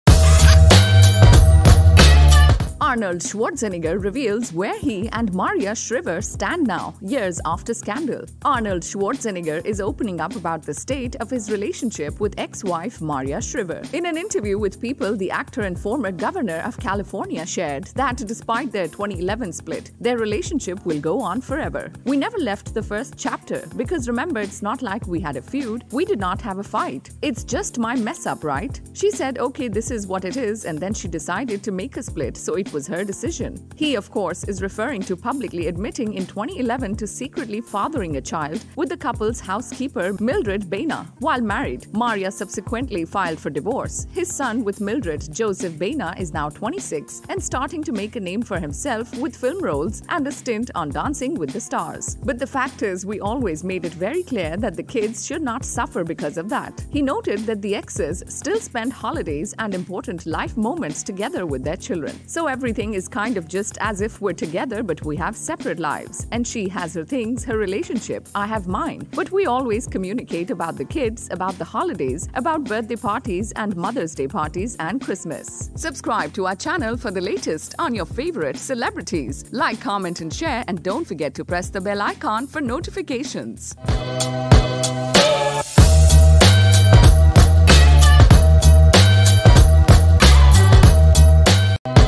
Arnold Schwarzenegger gives his two cents on relationship with Maria Shriver
Arnold Schwarzenegger can never forget Maria even after years of split.
His love for her Is alive and will always be

Arnold Schwarzenegger | Maria Shriver | Relationship